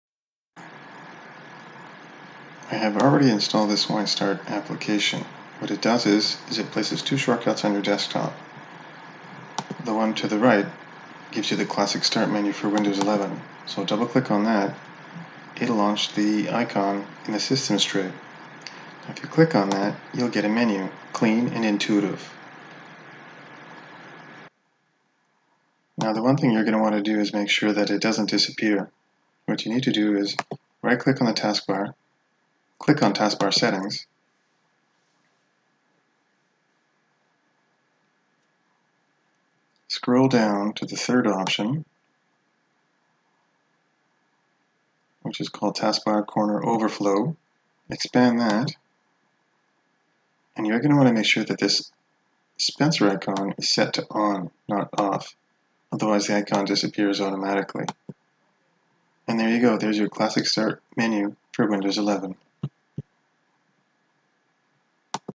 Classic Start Menu for Win 11
Wine Start Menu can provide a classic start menu for Win11 and, it can also be used in all versions of Windows.

Wine Start Menu can also be used on any OS that has Wine (winehq.org) already installed and working.

How-To install Wine in Ubuntu Linux
     - copy all of the text below and paste into Terminal, then click Enter
     - ​this is for a fresh install of Wine only
     - you must uninstall any current version of Wine